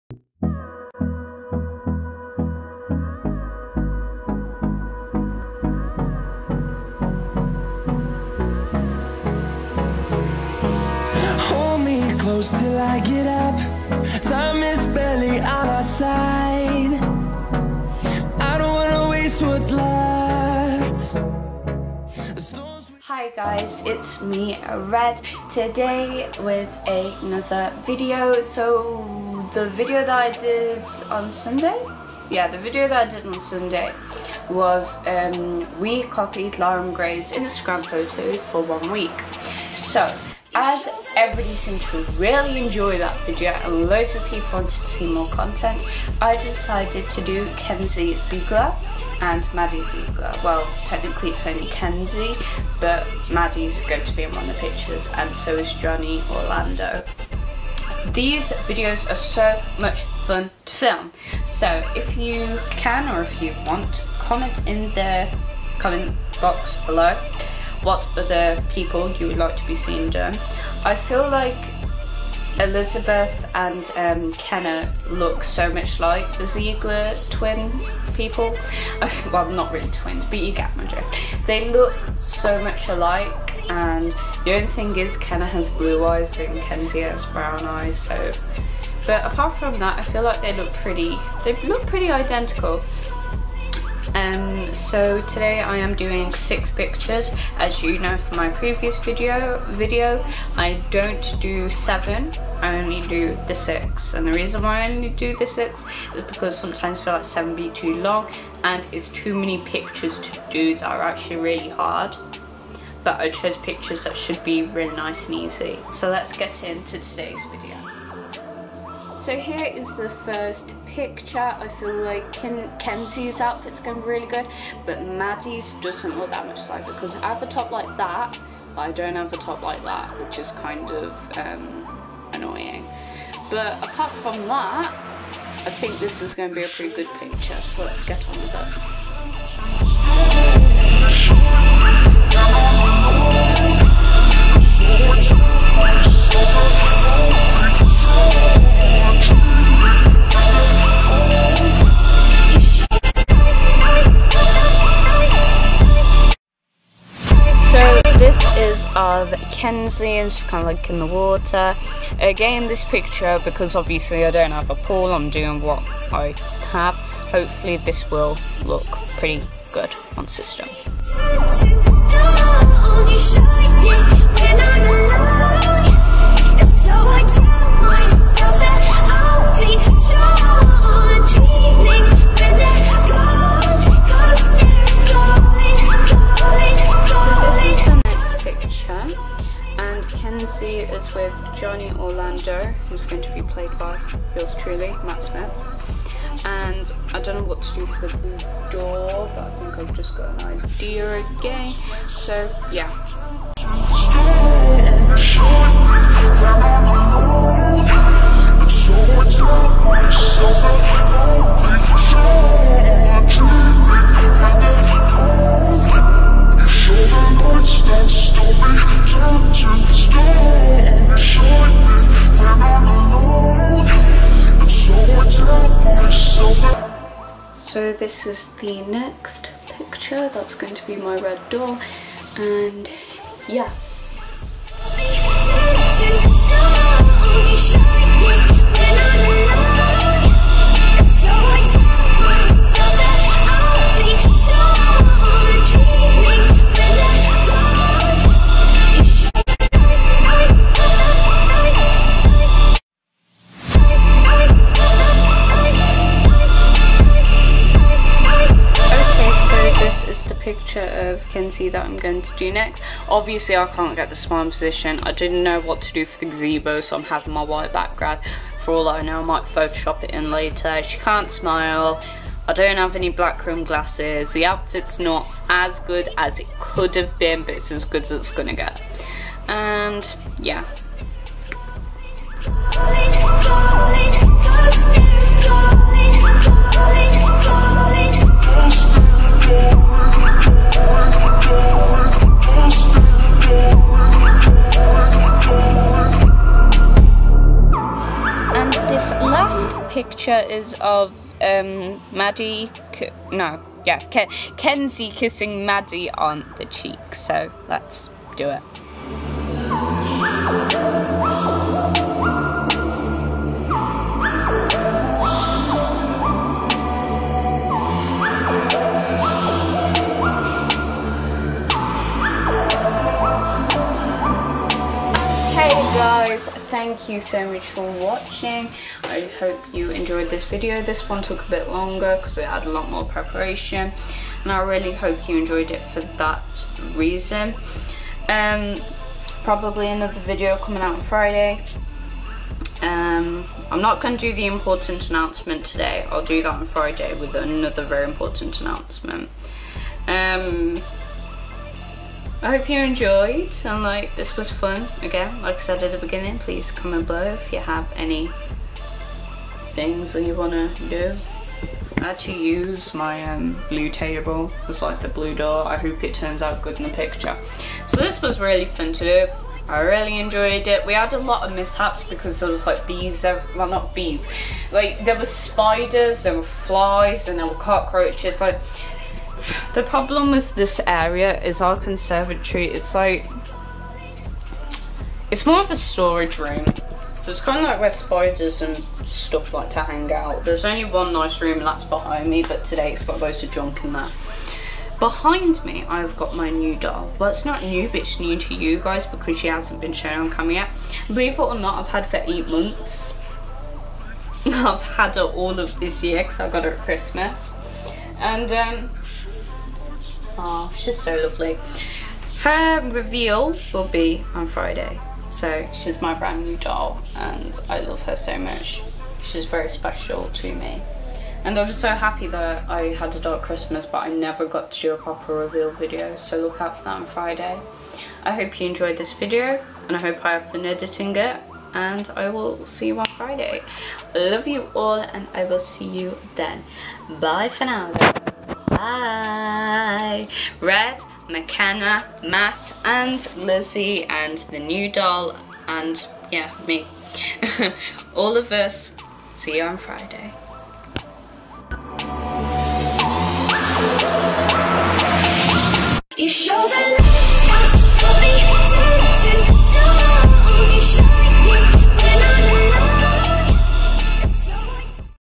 We Copied Kenzie Ziegler's Instagram Posts For A Week! AGLF
Hi guys!
Again, please remember that these are dolls and not humans!!
Soooooo thank you for 70 plus views on my previous video! This technically took two days to edit because I tried to photo shop the backgrounds in and. well it didn't go well so I left that out! XD
- Red
(who is coming back to agtube full time)